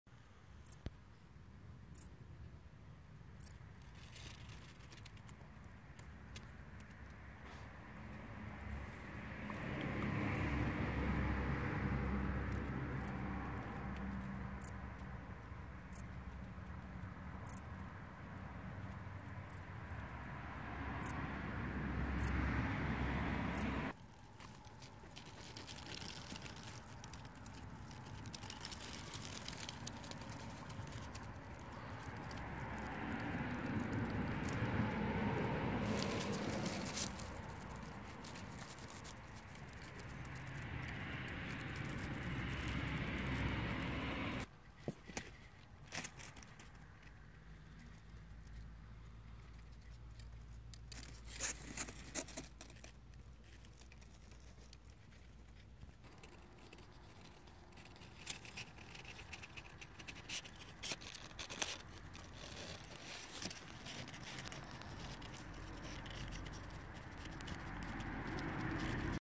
Zamaskowane konik polny
NIELE SIĘ UKRYWA  ,TROCHĘ GO SŁYCHAĆ.